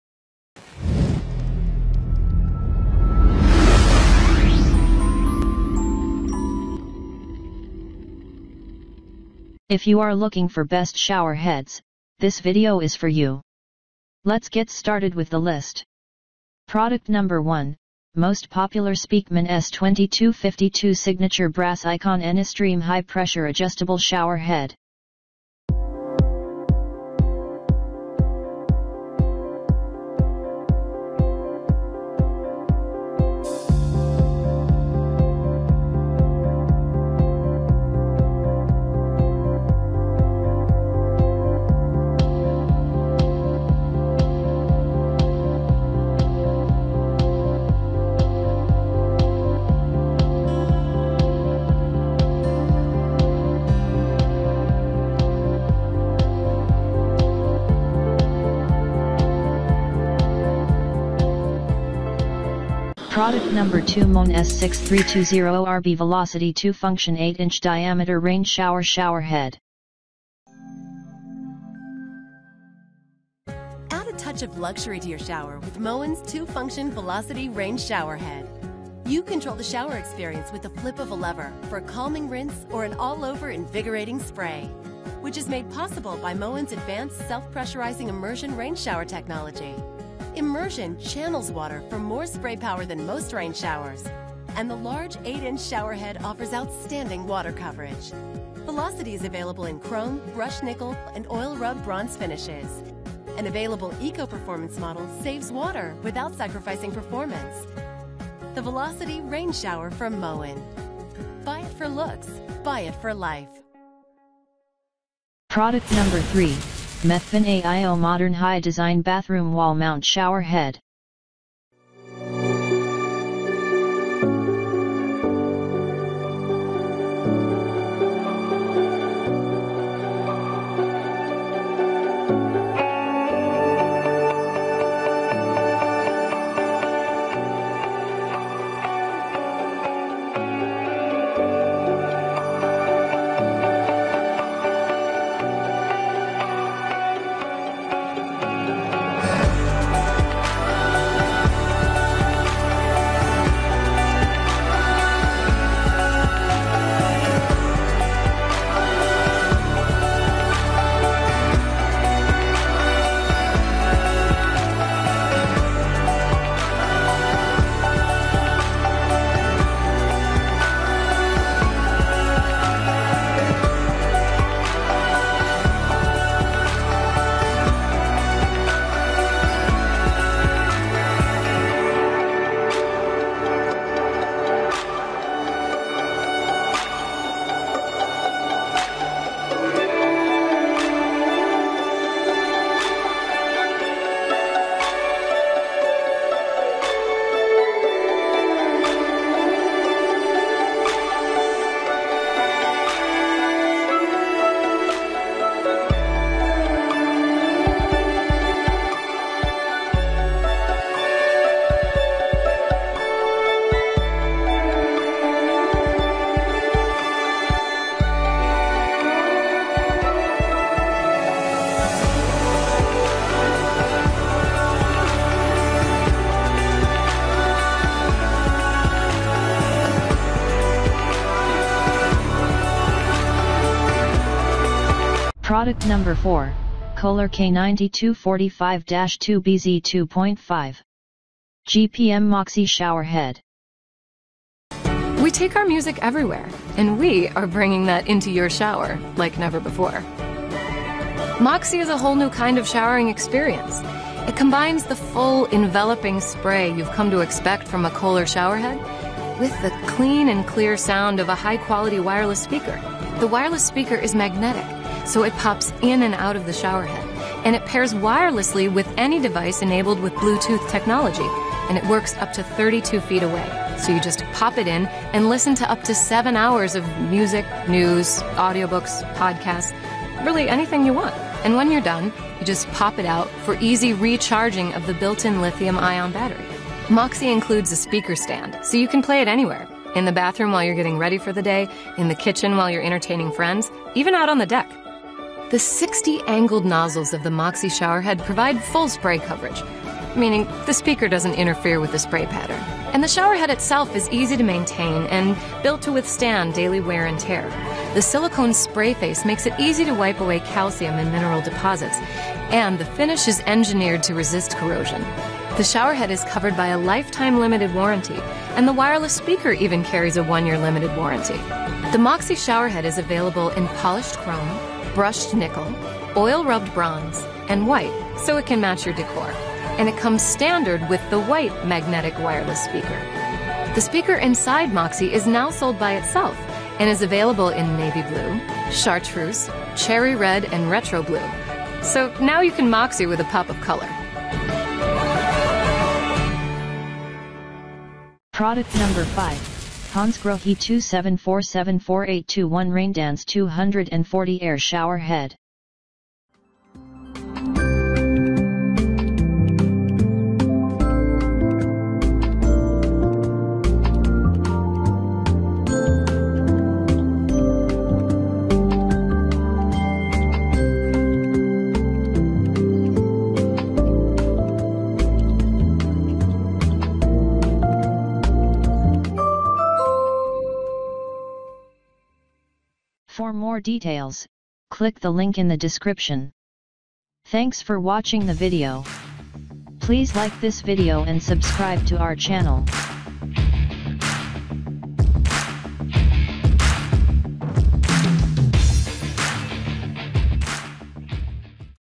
Top 5 Best Shower Heads
► Best Shower Heads

► 1. Speakman S-2252 Signature Brass Icon Anystream High Pressure Adjustable Shower Head
► 2. Moen S6320ORB Velocity Two-Function 8-Inch Diameter Rainshower Showerhead
► 3. Methven Aio Modern Hi-Tech Design Bathroom Wall Mount Shower Head
► 4. KOHLER K-9245-2BZ 2.5 GPM Moxie Showerhead
► 5. Hansgrohe 27474821 Raindance 240 Air Shower Head